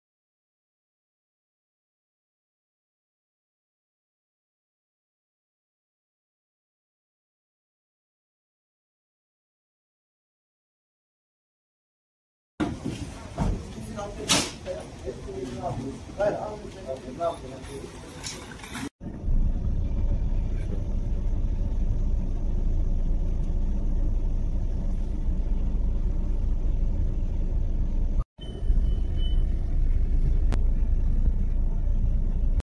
İnegöl'de Motosiklet Kazası: Güvenlik Kamerasına Yansıdı
İnegöl'de motosikletin yayaya çarptığı kazada 2 kişi yaralandı. Kaza, iş yerinin güvenlik kamerasına saniye saniye yansıdı.